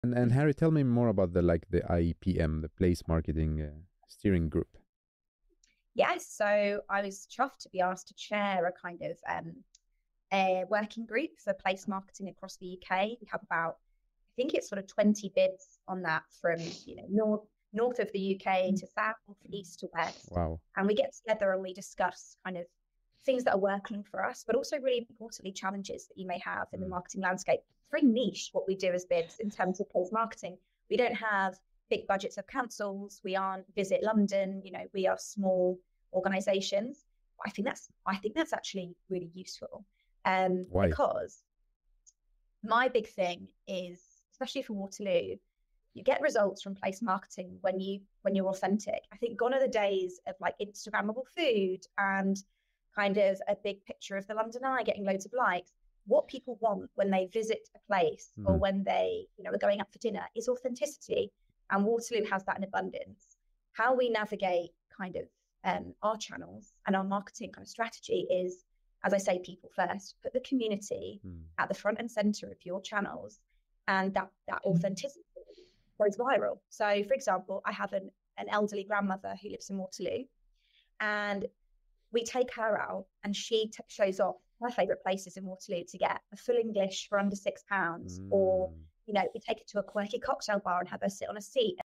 Discover Authentic Places in Waterloo: A Guide to Local Favorites and Hidden Gems
Join us as we explore the authentic side of Waterloo, away from the Instagram hype. From affordable full English breakfast spots to quirky cocktail bars, we'll share our grandmother's favorite hidden gems. Learn how to create a community-driven marketing strategy and go viral with authenticity.